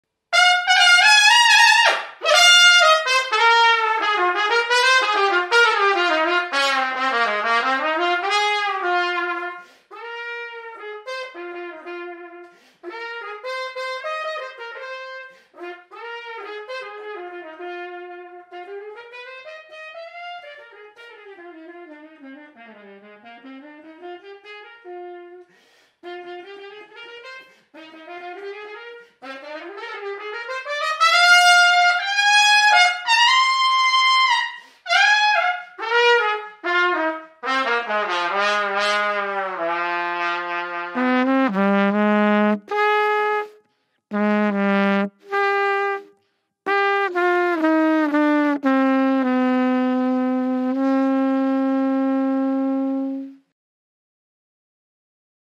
Mute Tube - Demonstration of functionality / damping performance - Models Basic + Professional
Practice & Record without disturbing your neighbours!

Mute Tube Basic - practice mute for trumpet & flugelhorn: a small sound booth for home use
Mute Tube Professional - practice and recording booth for trumpet & flugelhorn

Listen to audio tracks recorded inside Mute Tube Professional with several microphones

- four times volume reduction up to 25 dB
- free airflow through the instrument
- pure response and intonation
- easy installation of microphone

Practice or record music freely without disturbing your family and neighbours!

MUTE TUBE - FEEL THE REAL SOUND